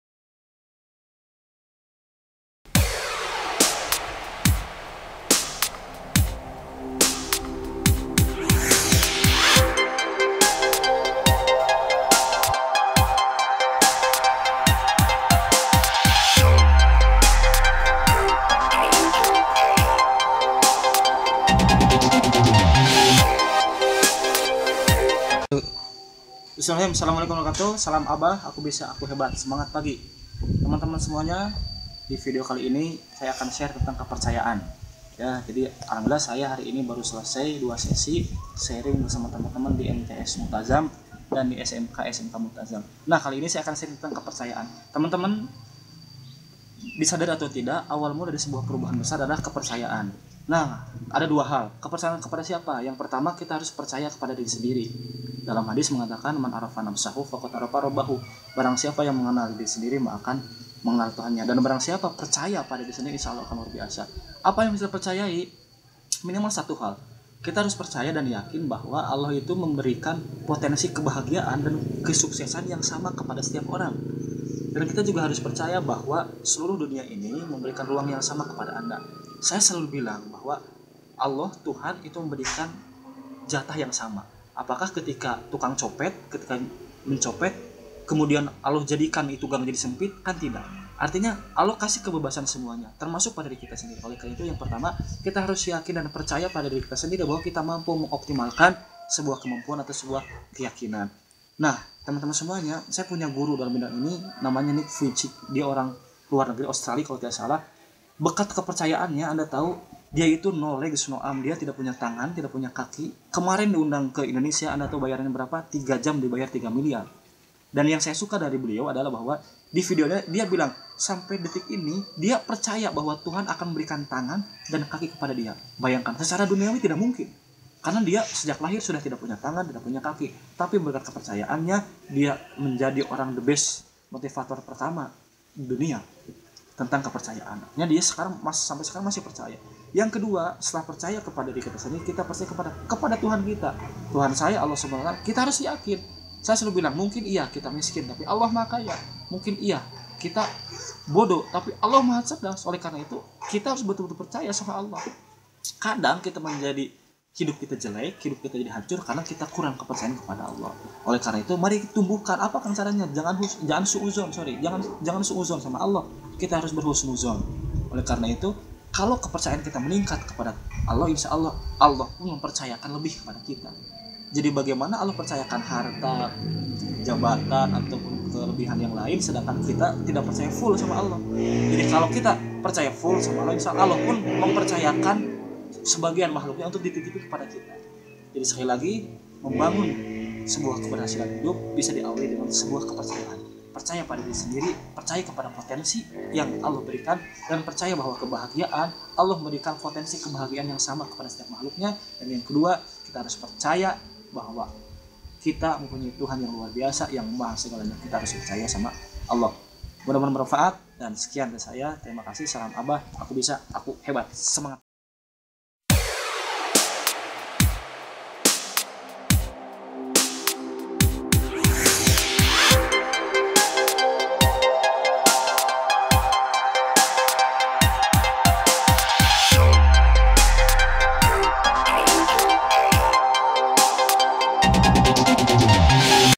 PENTINGNYA SEBUAH KEPERCAYAAN
Orang yang hebat adalah ia yang percaya bahwa Allah SWT memberikan potensi kebahagiaan yang sama kepada semua manusia. Mari bangun kepercayaan itu!